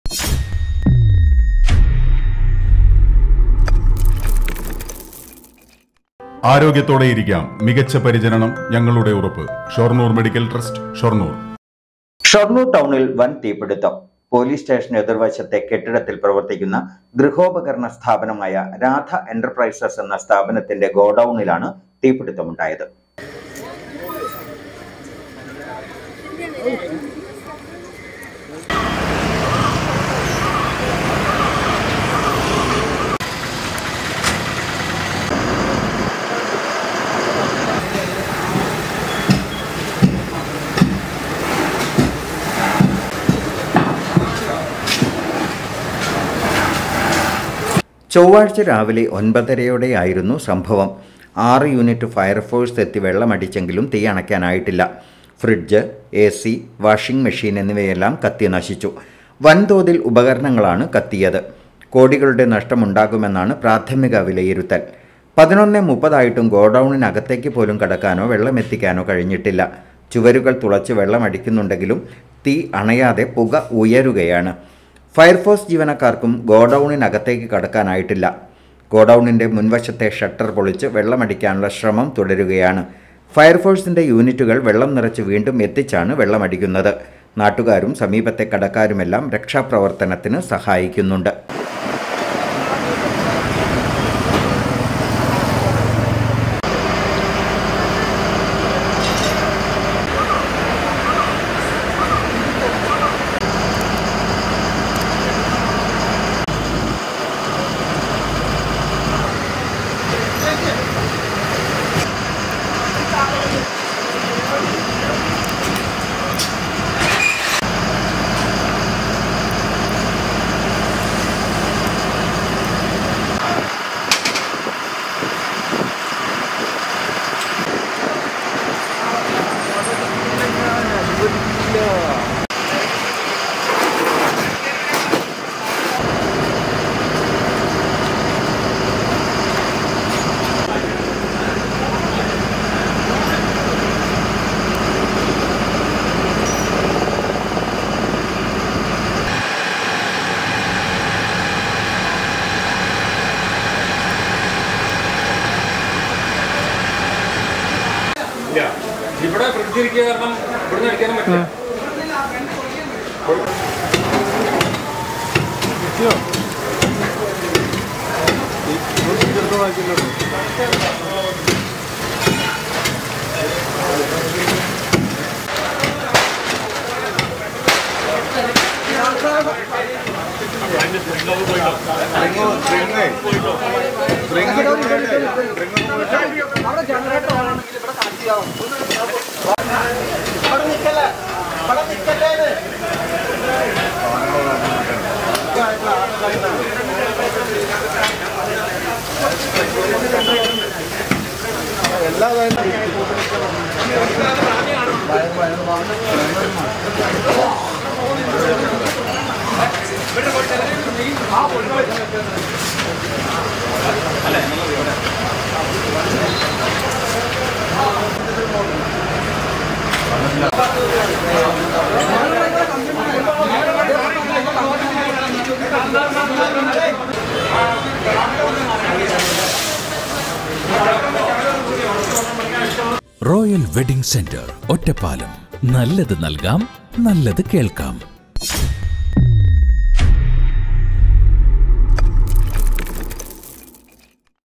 ഷൊർണൂർ ടൗണിൽ വൻ തീപിടുത്തം. രാധാ എന്റർപ്രൈസസ് ഗൃഹോപകരണ സ്ഥാപനത്തിന്റെ ഗോഡൗണിലാണ് തീപിടുത്തമുണ്ടായത്.
ഷൊർണൂർ ടൗണിൽ വൻ തീപിടുത്തം. പോലീസ് സ്‌റ്റേഷന് എതിർവശത്തെ രാധാ എന്റർപ്രൈസസ് എന്ന ഗൃഹോപകരണ സ്ഥാപനത്തിന്റെ ഗോഡൗണിലാണ് തീപിടുത്തമുണ്ടായത്.